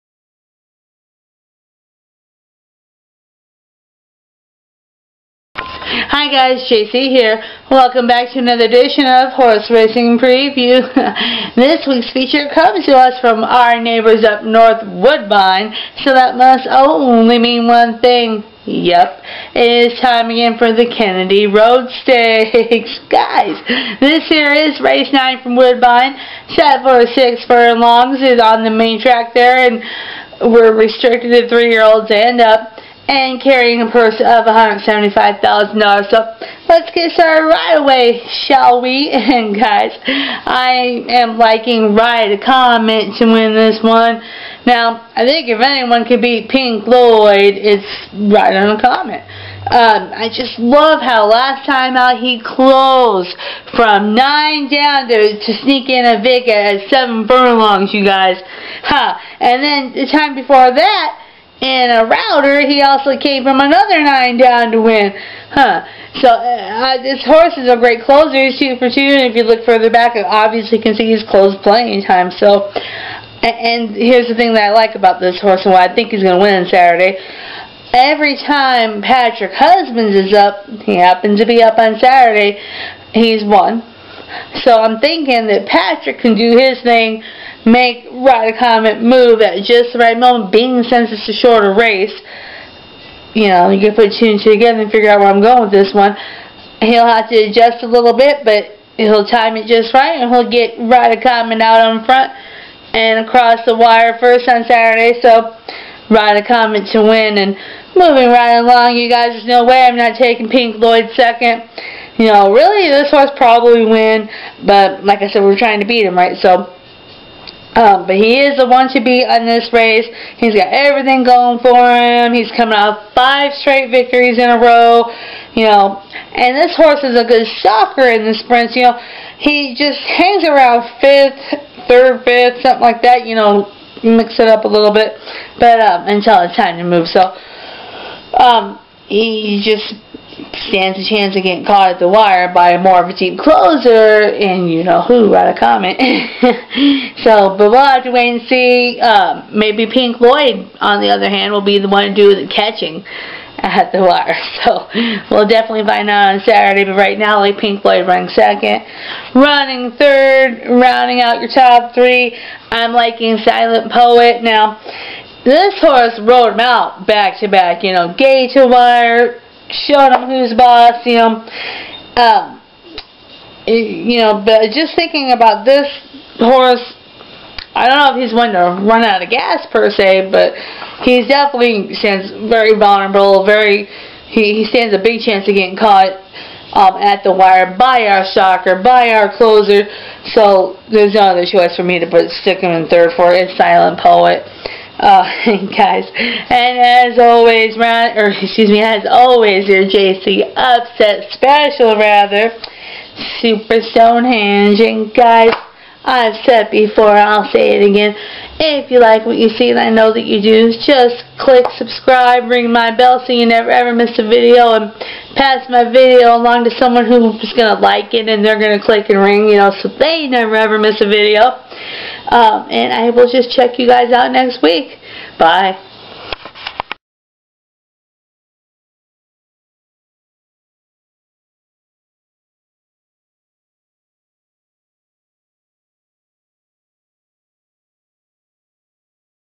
Horse Racing Preview! Kennedy Road Stakes 2020!
Hay guys, hay!  This is the Kennedy Road Stakes.  Race 9 from Woodbine!  6 furlongs, 3-year-olds and up, $175,000 purse!

The field:
Souper Stonehenge  (FL)  12/1
Ride a Comet  (KY)  5/2
Eskiminzin  (ON)  20/1
Silent Poet  (ON)  2/1
Dixie's Gamble  (ON)  15/1
Roaring Forties  (FL)  20/1
Pink Lloyd  (ON)  7/5